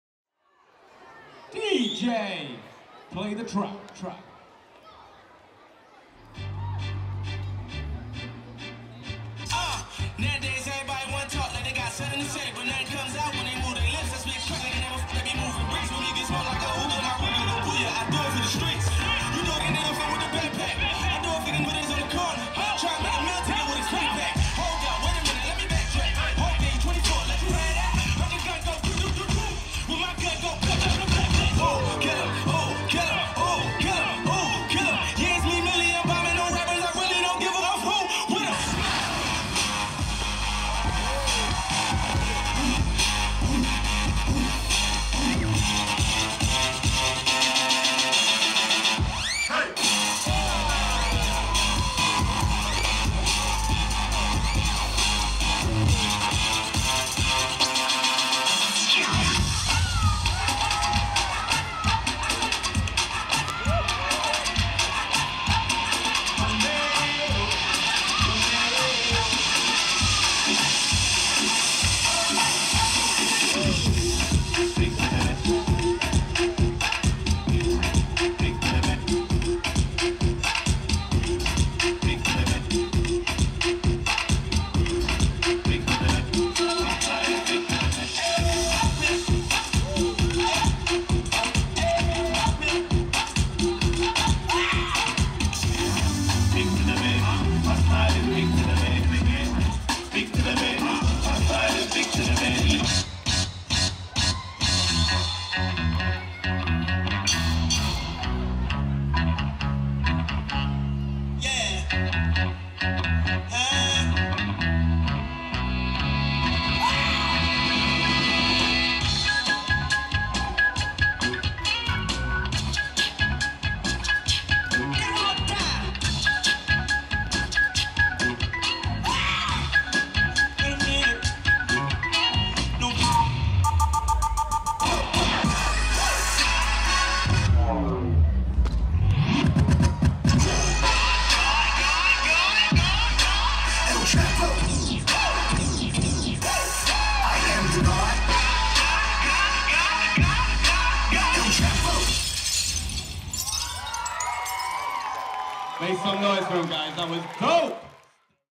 Stereoshok - 14 & Under Open - BDO South Coast 2016
Team performance at the BDO South Coast Championships Sunday 1st May 2016 at Worthing Leisure Centre.